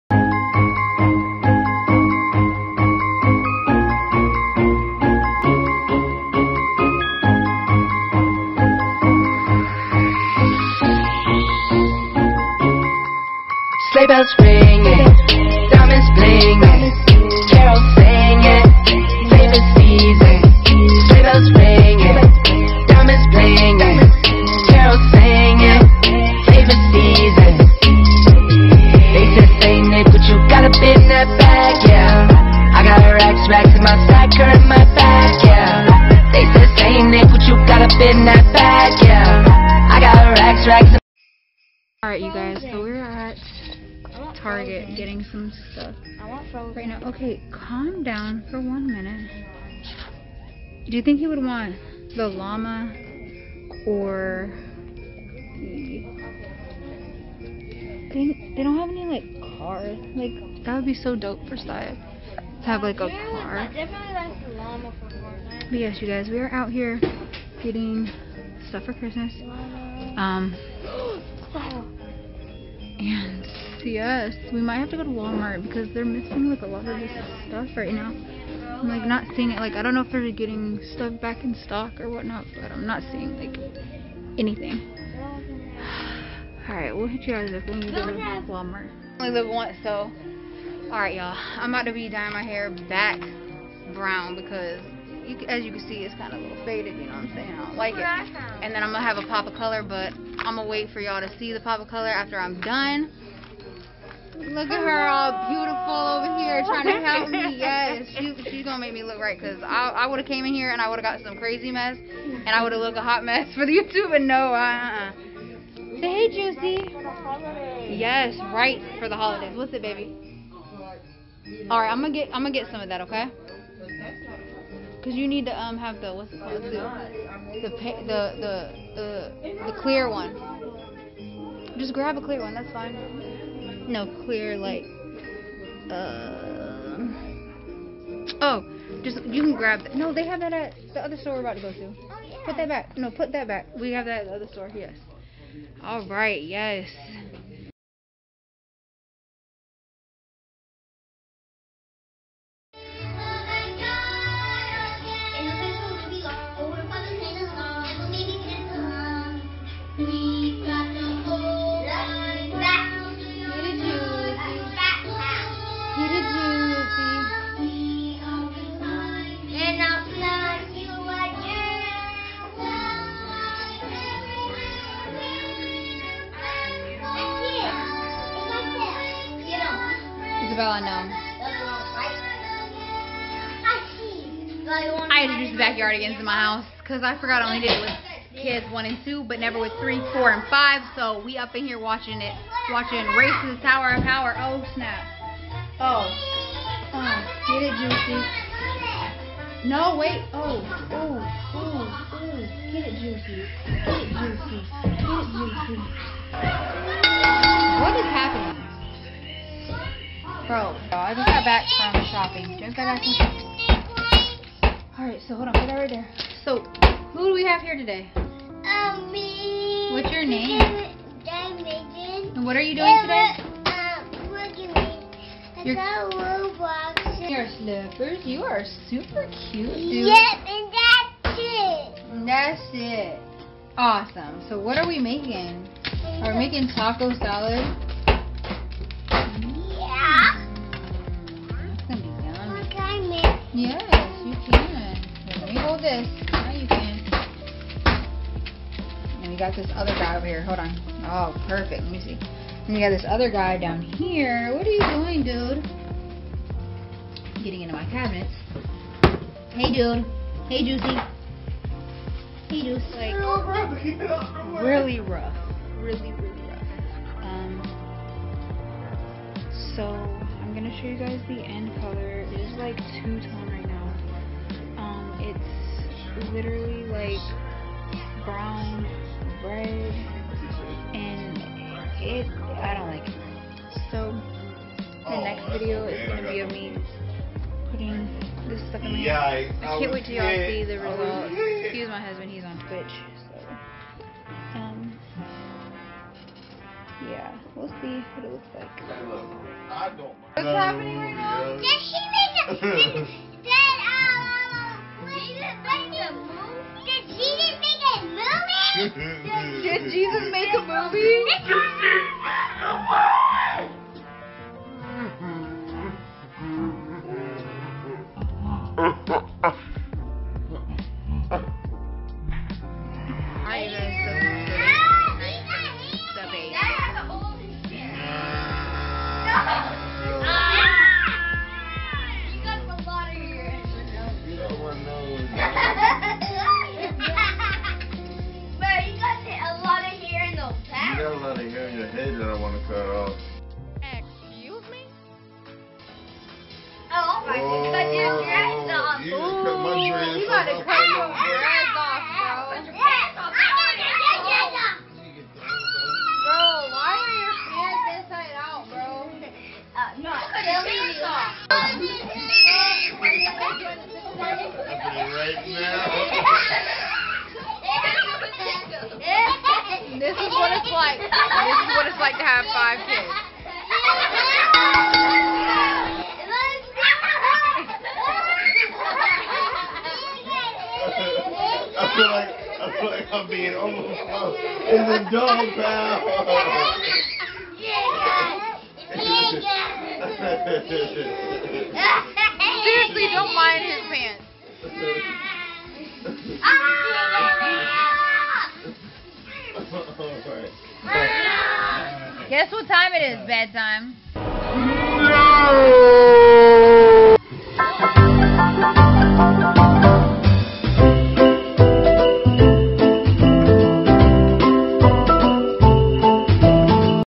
(Vlogmas Day 3) New Hair Color| WWE Fighting all day (Hilarious)
Its Christmas month with The Porters , I decided that I wanted to try something different with my hair. We Did a little Christmas shopping , cooking with a special guest and a little love fighting .

Family of 7 , Just having fun.